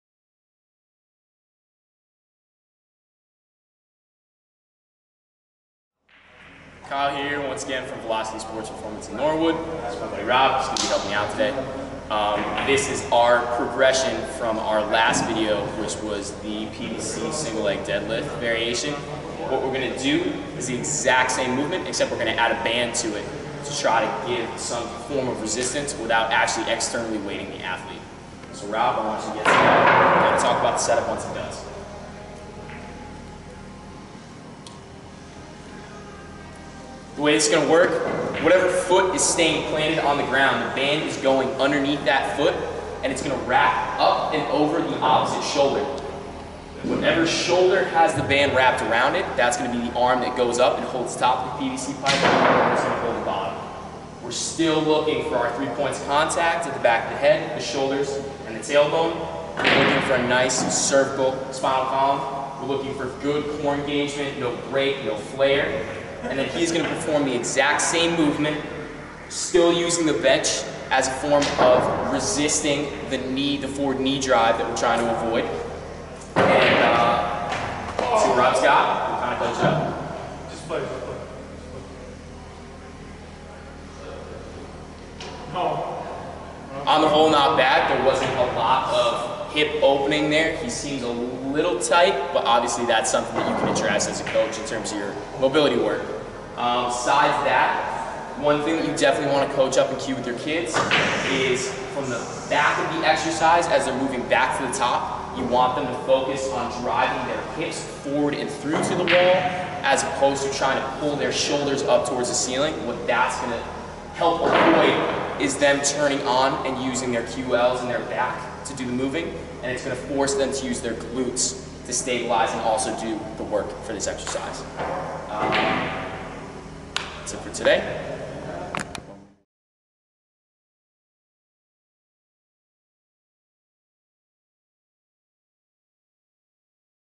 Hip Hinge progression #3
Coach Kyle Ashworth continues his series on the progression of the Hip Hinge.  A staple of any Strength Program